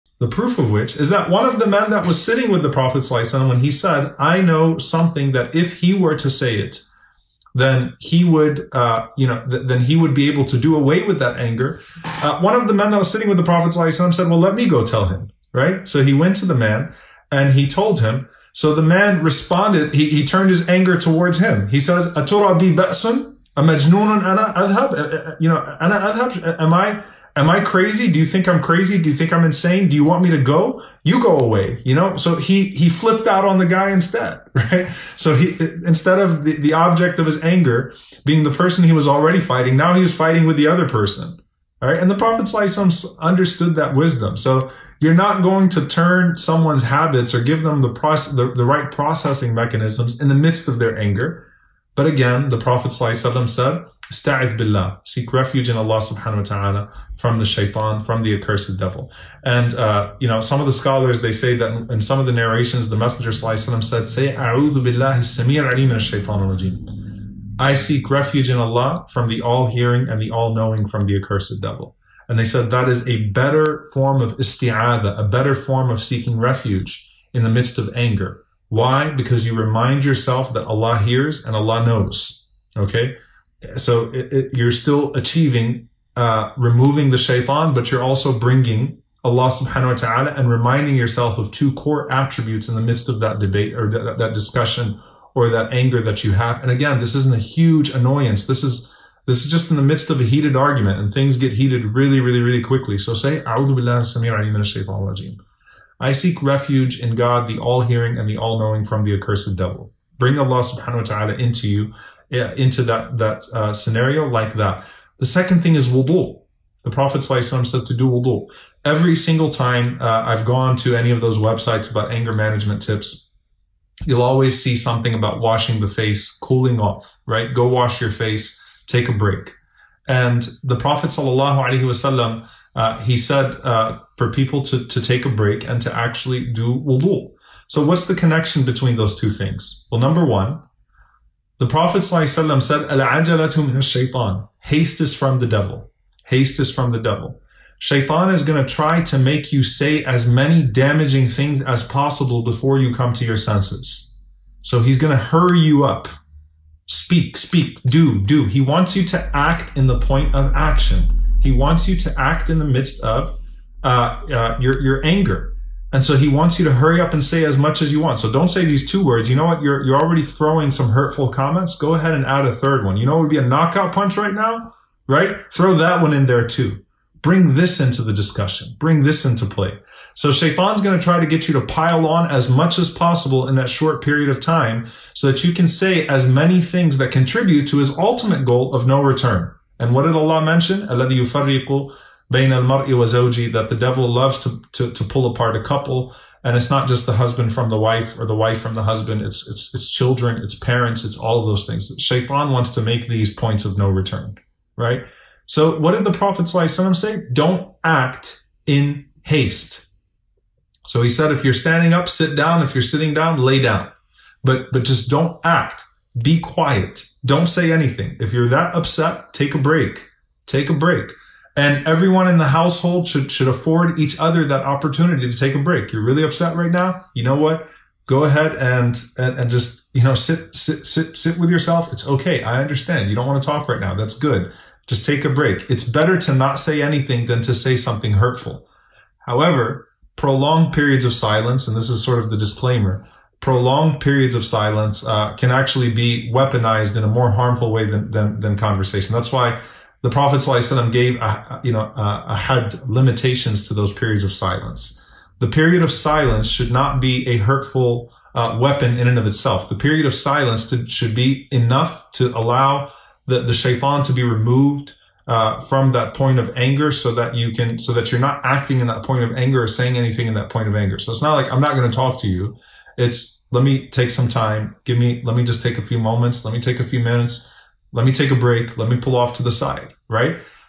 Control Your Anger - Dr. Omar Suleiman
While being under lockdown, often we can become frustrated and easily irritated. Sh. Omar Suleiman discusses maintaining peace at home and in general in stressful times.